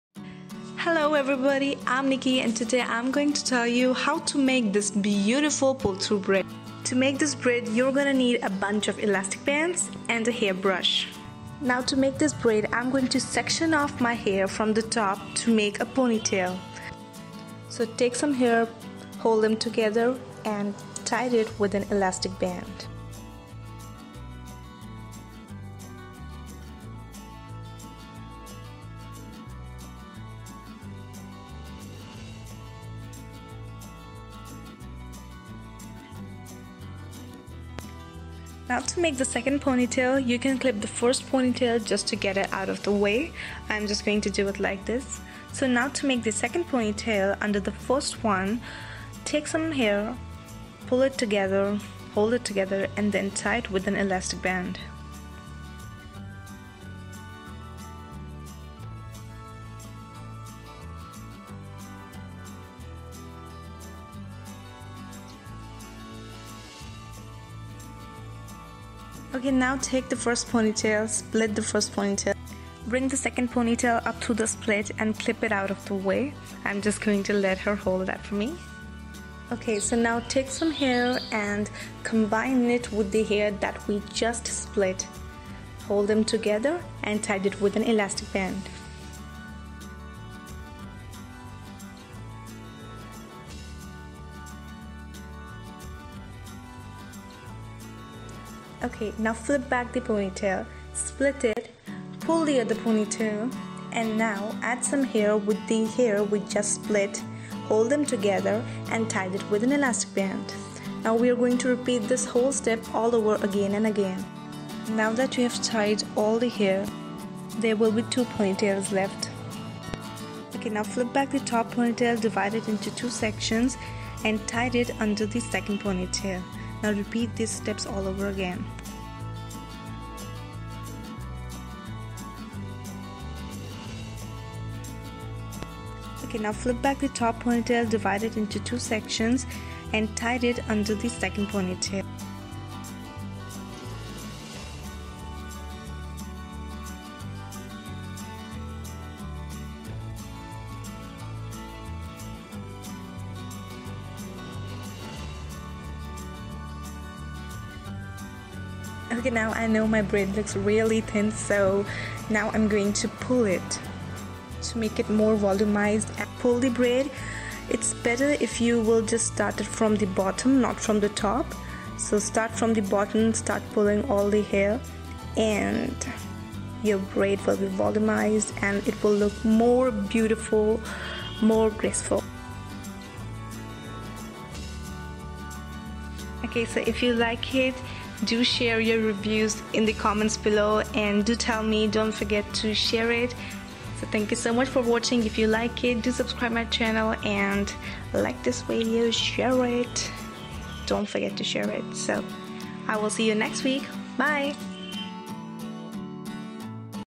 How to: pull through braid|simple and easy braid hairstyle
A simple pull through braid tutorial. Thumbs up if you liked it.Where i live its really hot in here,so in i really like to do this braid . This pull-through braid is very quick, especially once you get the hang of it. I love how it gives the illusion of having a very voluminous,thick  pony tail :) .
 It is great for all hair types, whether you have short hair, long, or layered this will work.
xoxo